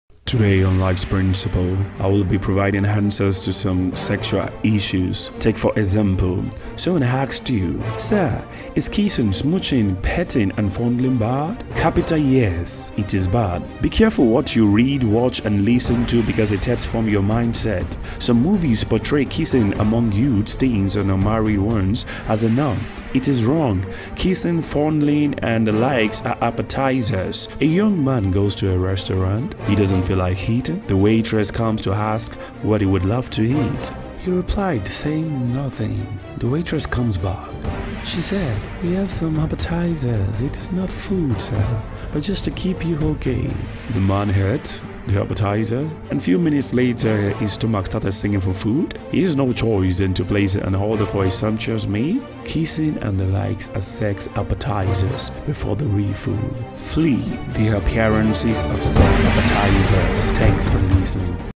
SEX APPETIZER
learn how to deal with sex appetizers. sex is not the problem but APPETIZERS. Appetite can never be satisfied but it can be controlled